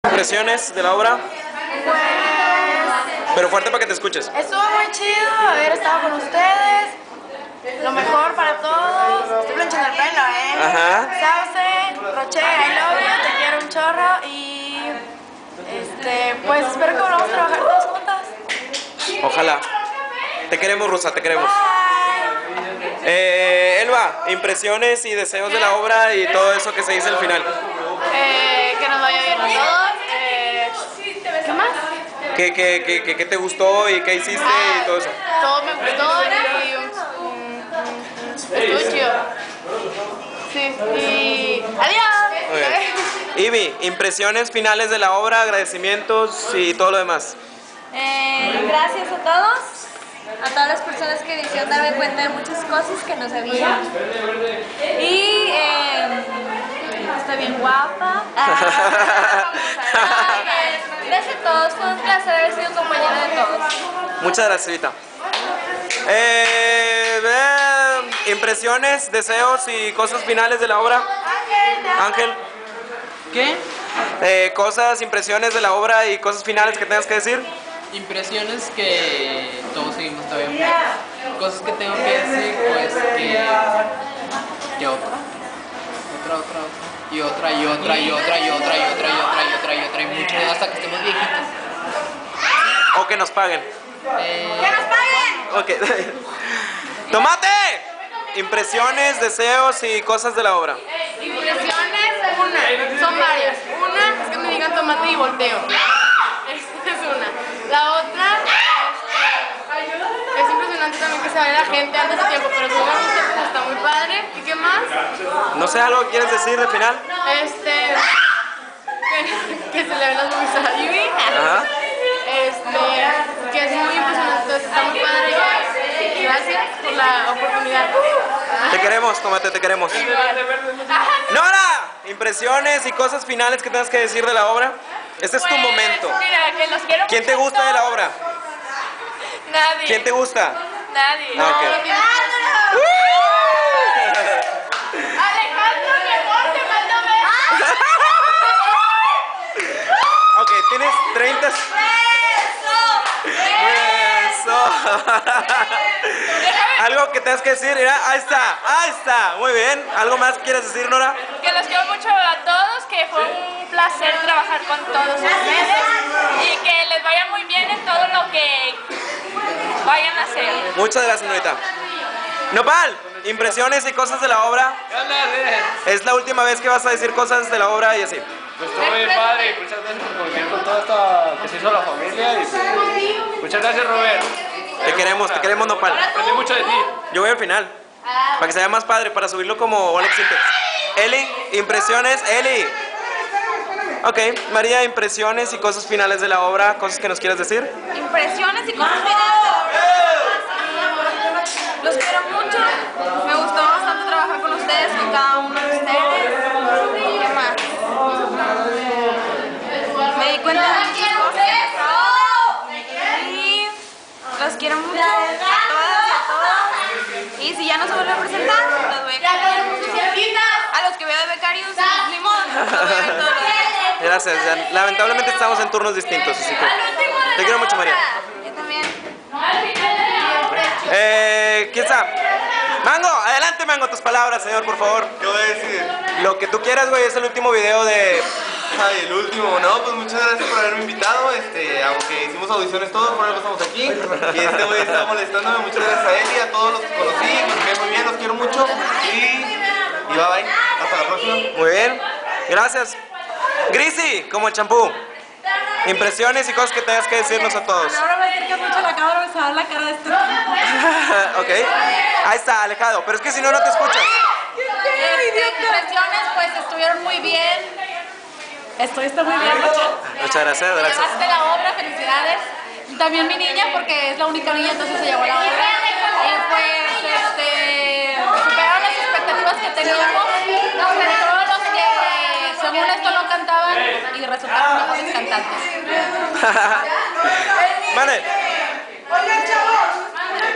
Comentarios Finales!! (1)
De la última presentación de Qué Plantón! todos los comentarios, las despedidas, las declaraciones amorosas y "las cosas esas que dice la gente cuando se despide".